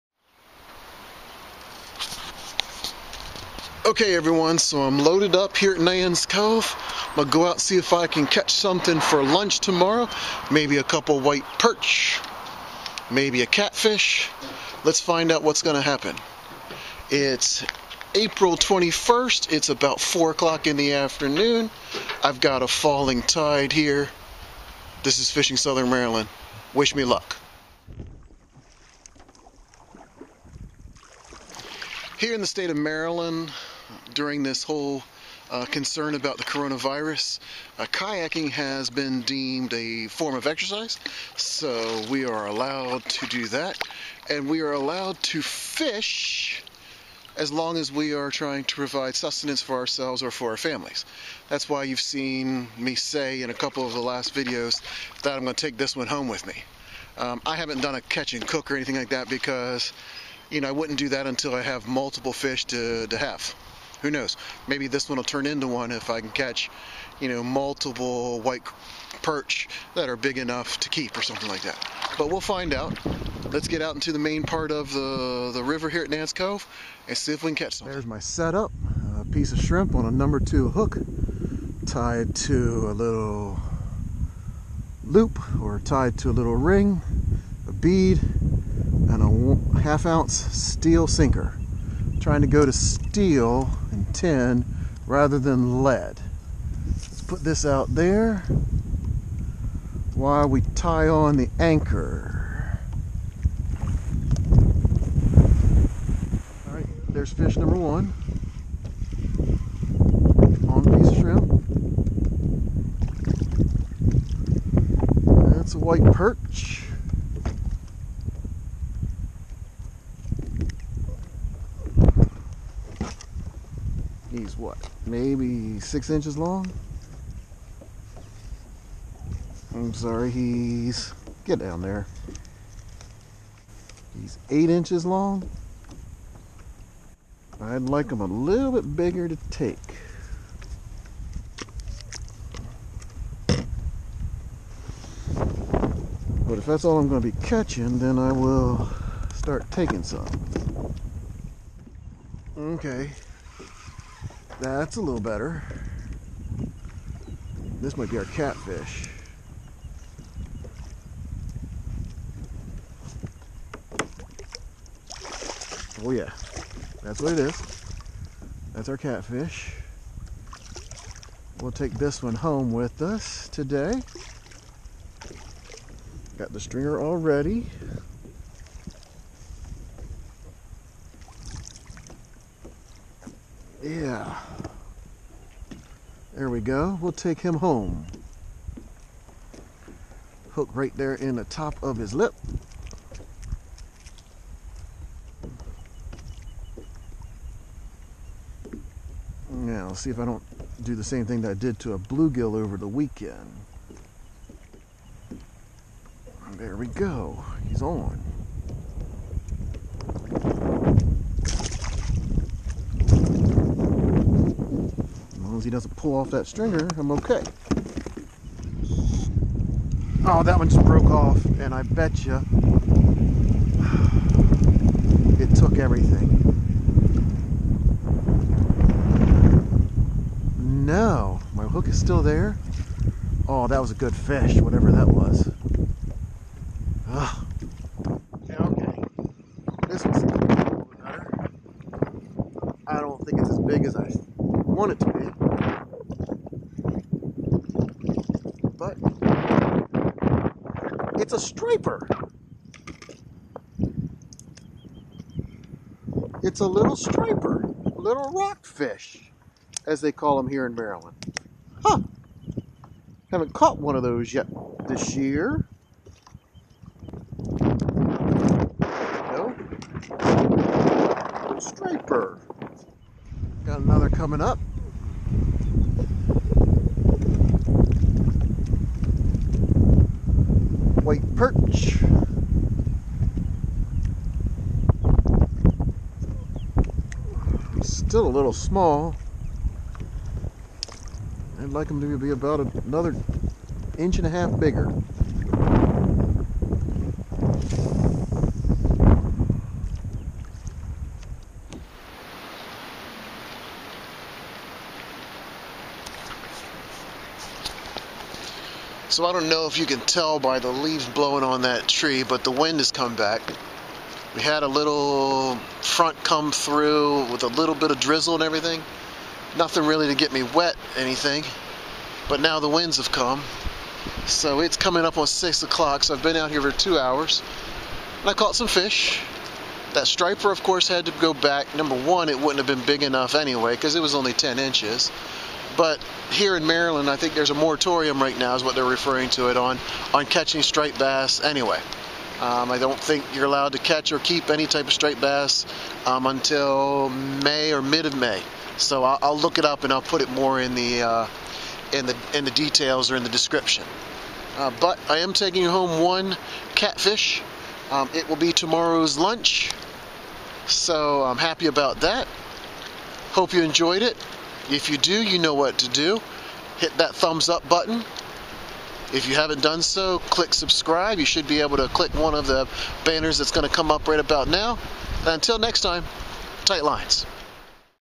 What's Biting at Nan's Cove? (April 21, 2020)
From April 21, 2020: I took the kayak over to Nan's Cove on the Patuxent River earlier this week in search of something to bring home.  Using my standard pieces of shrimp I was able to catch three different species on the falling tide.  Pardon the wind noise as a system came through near the end of my trip but I did find some fish!  Enjoy!